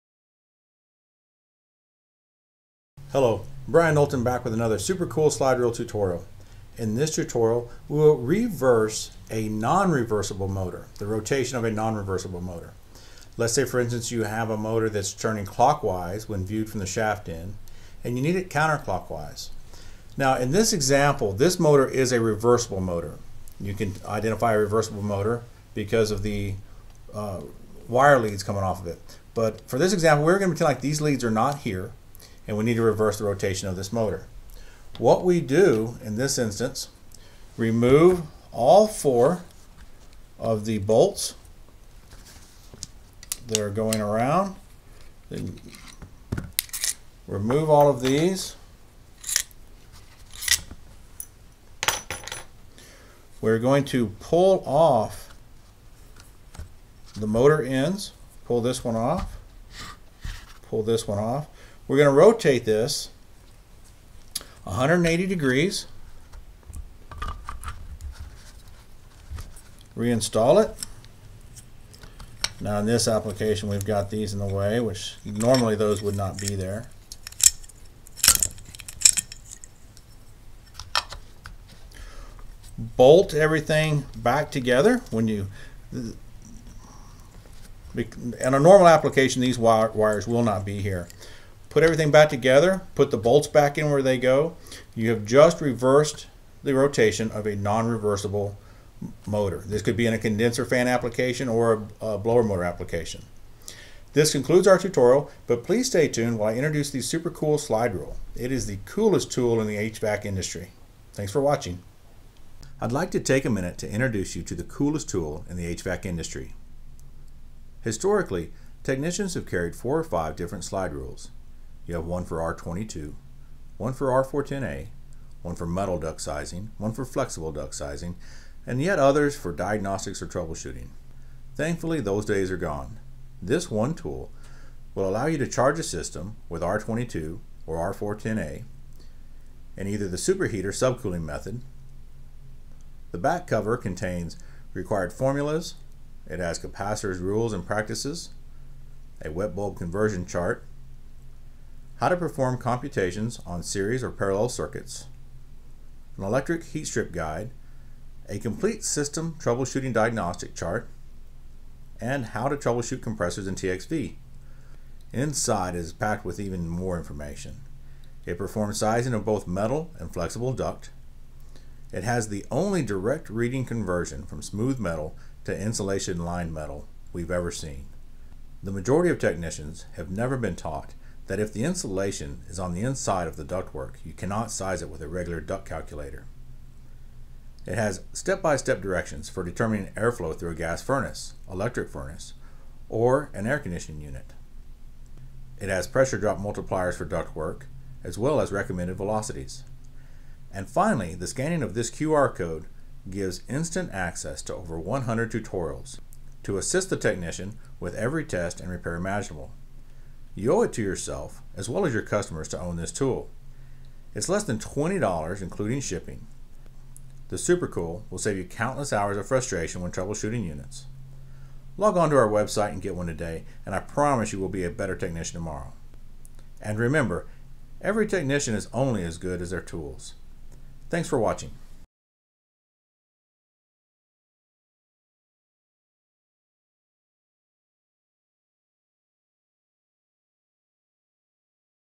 How to change the Rotation of a Non-reversible Motor
-- This video tutorial demonstrates how to simply reverse the rotation of a non-reversible motor. Reversing the rotation of a non-reversible motor is simple using these step-by-step directions.